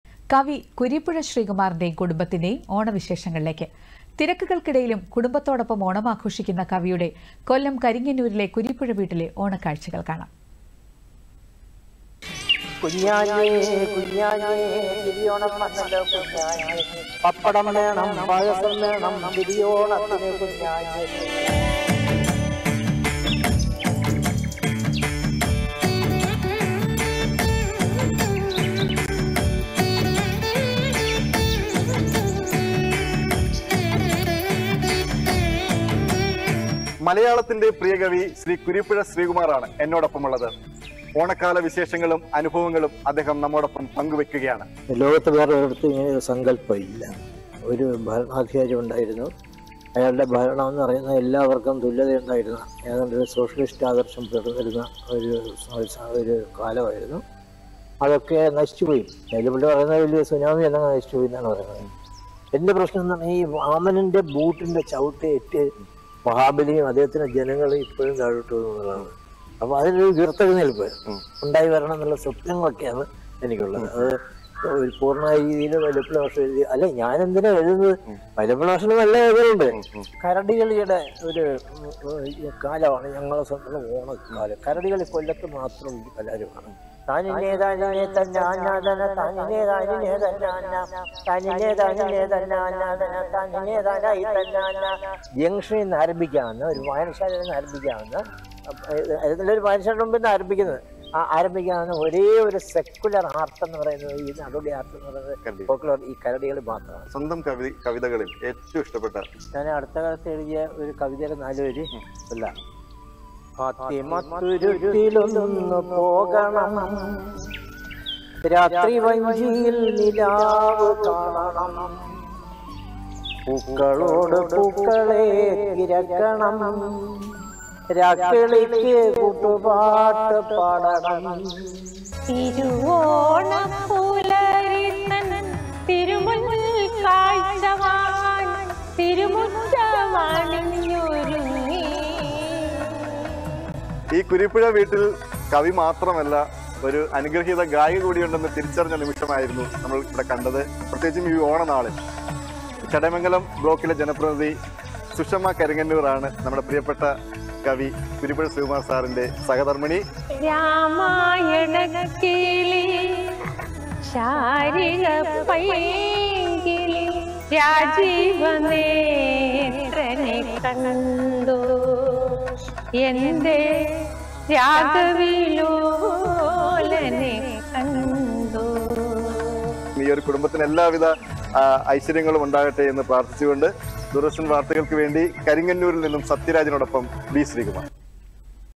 കവി കുരീപ്പുഴ ശ്രീകുമാറിന്റെയും കുടുംബത്തിന്റെയും ഓണവിശേഷങ്ങൾ
കവി കുരീപ്പുഴ ശ്രീകുമാറിന്റെയും കുടുംബത്തിന്റെയും ഓണവിശേഷങ്ങളിലേക്ക്.

തിരക്കുകൾക്കിടയിലും കുടുംബത്തോടൊപ്പം ഓണം ആഘോഷിക്കുന്ന കവിയുടെ കൊല്ലം കരിങ്ങന്നൂരിലെ കുരീപ്പുഴ വീട്ടിലെ ഓണ കാഴ്ചകൾ കാണാം.